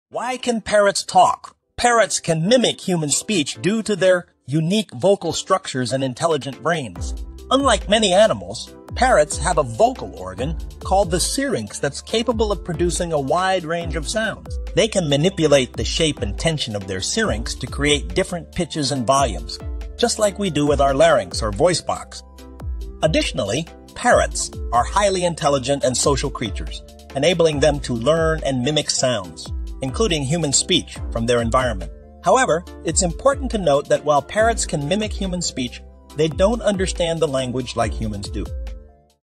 Why can parrots talk?
Let's explore the marvel of parrots' ability to talk! 🦜🗣️ Delve into their unique vocal structures, such as the incredible syrinx, which grants them the power to mimic a wide range of sounds, including human speech. Uncover the intelligence and social nature that contribute to their extraordinary imitative skills. 🌟🎙️